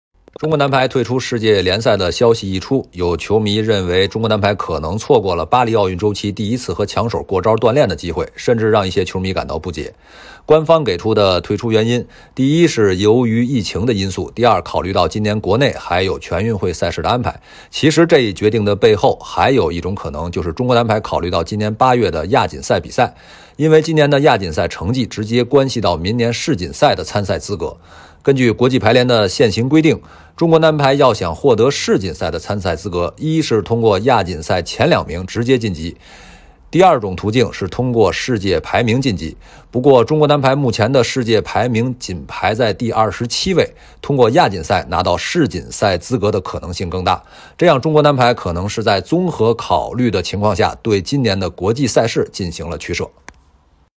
记者解读：中国男排退出世界排球联赛或因保亚锦赛
日前，国际排联发布公告称，因疫情造成的财务和旅行限制，以及14天隔离政策和全运会的安排冲突，中国排协决定退出5月底在意大利举行的世界男排联赛。对此，来听听中国中央广播电视总台记者王伟的观点。